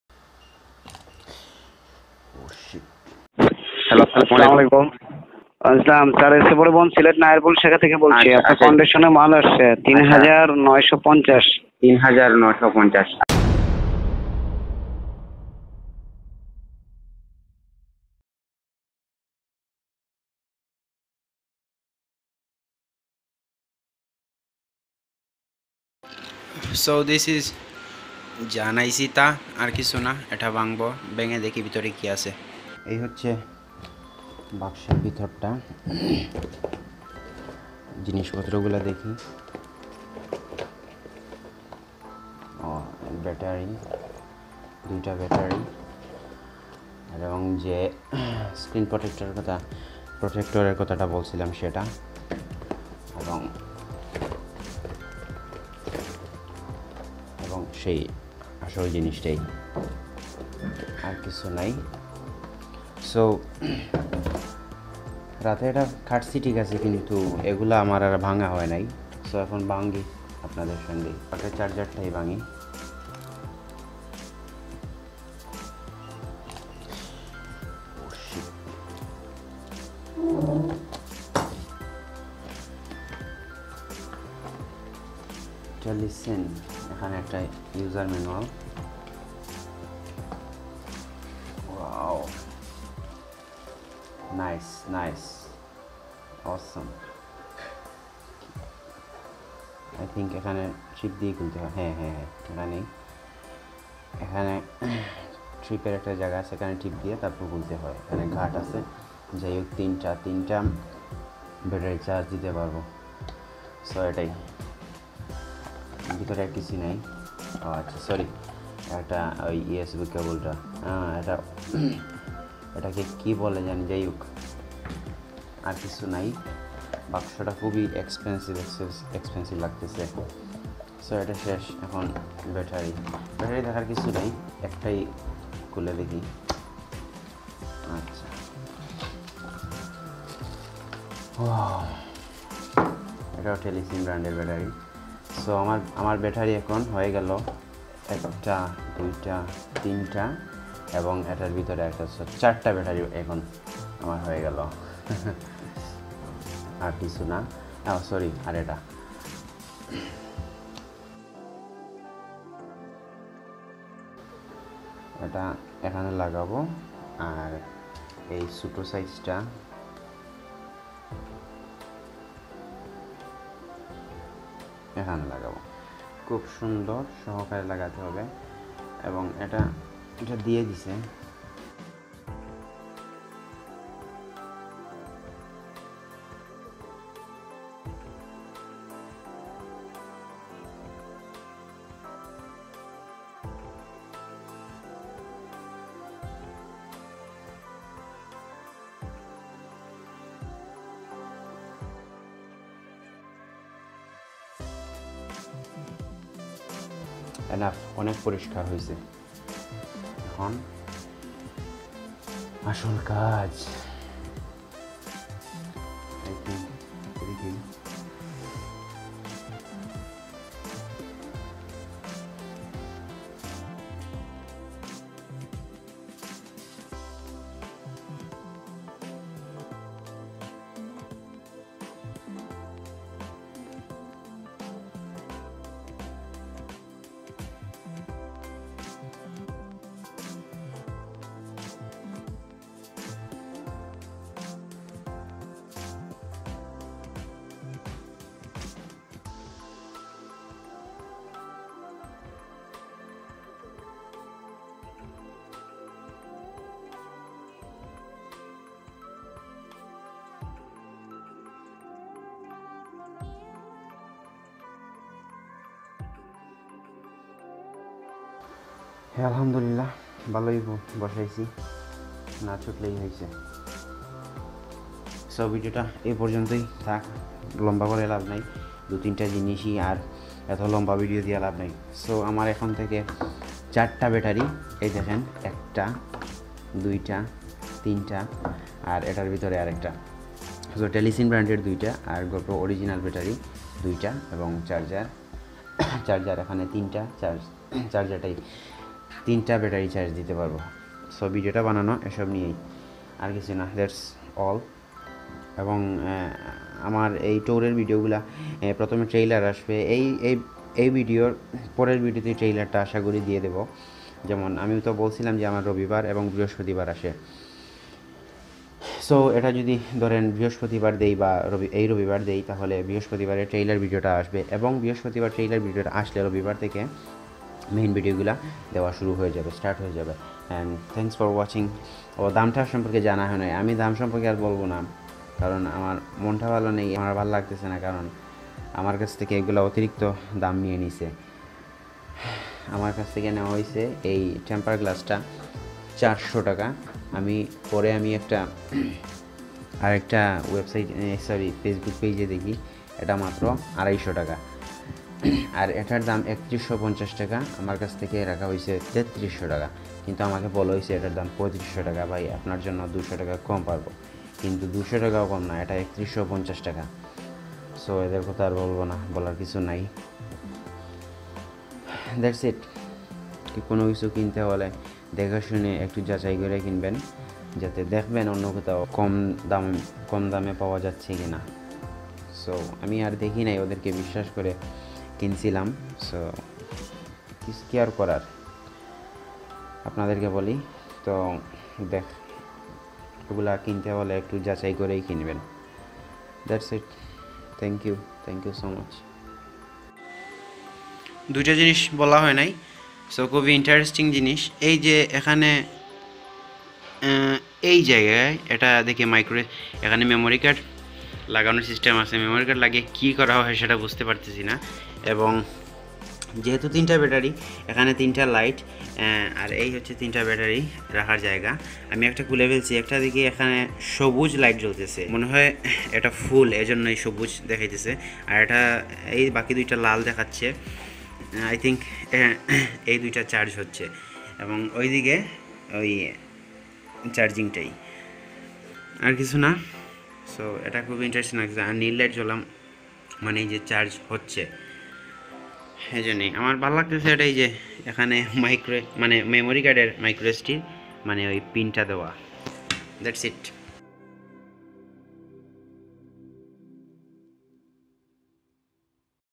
Telesin Waterproof Charger+Two Battery | LCD Screen Protector & Lens Protector | Avenue Tourist
The Items are:1) Telesin Waterproof Charger+Two Battery. Price; 3300/-2) LCD Screen Protector & Lens Protector. Price; 400/-For GoPro Hero 7 Black